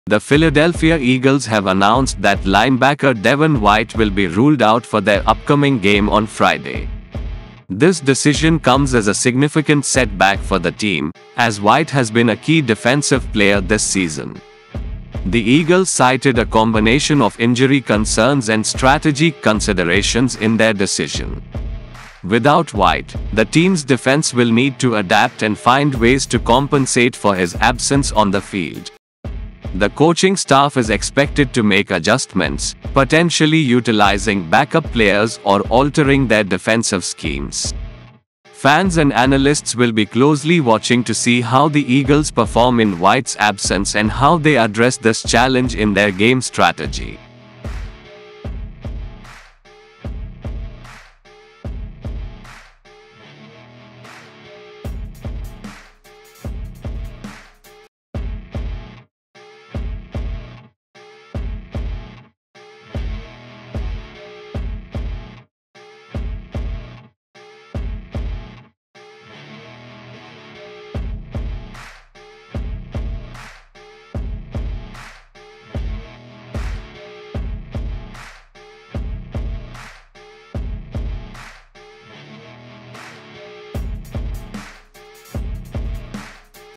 Eagles rule Devin White out for Friday
The Philadelphia Eagles have announced that linebacker Devin White will be ruled out for their upcoming game on Friday. This decision comes as a significant setback for the team, as White has been a key defensive player this season. The Eagles cited a combination of injury concerns and strategic considerations in their decision. Without White, the team's defense will need to adapt and find ways to compensate for his absence on the field. The coaching staff is expected to make adjustments, potentially utilizing backup players or altering their defensive schemes. Fans and analysts will be closely watching to see how the Eagles perform in White’s absence and how they address this challenge in their game strategy.